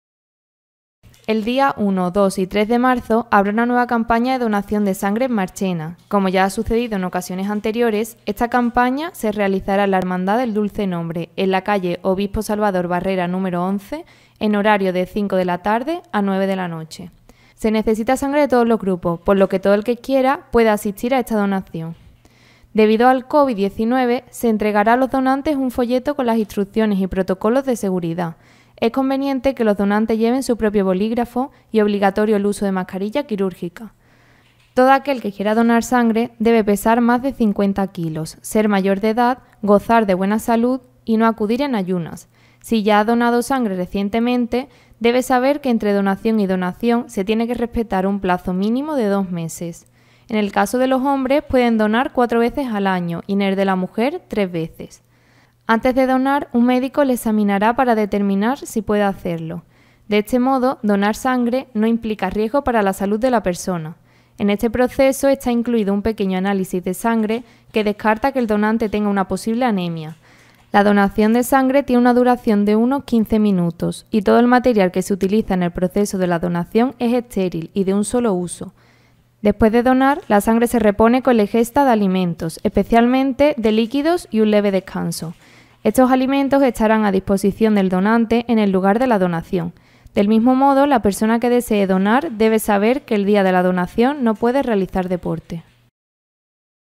Nueva campaña de donación de sangre en Marchena
PUBLICA RTVMARCHENA

El día 1, 2 y 3 de marzo habrá una nueva campaña de donación de sangre en Marchena. Como ya ha sucedido en ocasiones anteriores, esta campaña se realizará en la Hermandad del Dulce Nombre, en la calle Obispo Salvador Barrera nº 11, en horario de 17:00h a 21:00h.

Se necesita sangre de todos los grupos por lo que todo el que quiera puede asistir a esta donación.

Debido al COVID-19, se entregará a los donantes un folleto con las instrucciones y protocolo de seguridad. Es conveniente que los donantes lleven su propio bolígrafo y obligatorio el uso de mascarilla quirúrgica.

Todo aquel que quiera donar sangre debe pesar más de 50 kg, ser mayor de edad, gozar de buena salud y no acudir en ayunas. Si ya ha donado sangre recientemente, debe saber que entre donación y donación se tiene que respetar un plazo mínimo de dos meses. En el caso de los hombres pueden donar cuatro veces al año y en el de la mujer tres veces.

Antes de donar, un médico le examinará para determinar si puede hacerlo. De este modo, donar sangre no implica riesgo para la salud de la persona. En este proceso está incluido un pequeño análisis de sangre que descarta que el donante tenga una posible anemia. La donación de sangre tiene una duración de unos quince minutos y todo el material que se utiliza en el proceso de la donación es estéril y de un solo uso.

Después de donar, la sangre se repone con la ingesta de alimentos, especialmente de líquidos y un leve descanso. Estos alimentos estarán a disposición del donante en el lugar de la donación. Del mismo modo, la persona que desee donar debe saber que el día de la donación no puede realizar deporte.